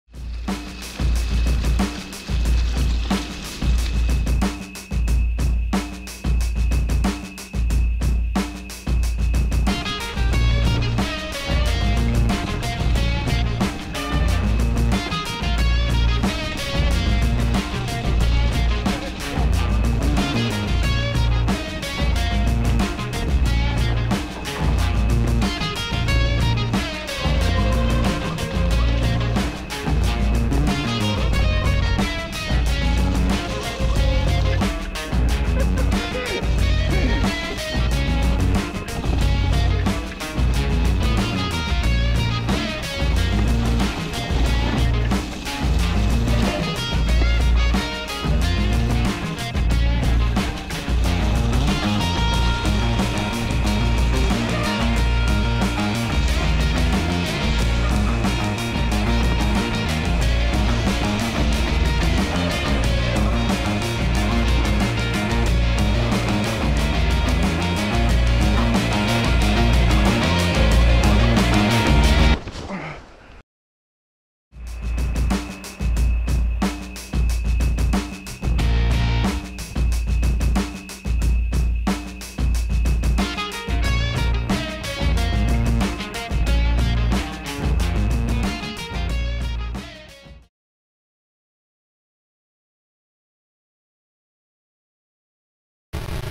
Enduro MTB Snow Ride | GoPro POV
Es war eine sehr rutschige Angelegenheit, aber es hat sehr viel Spaß gemacht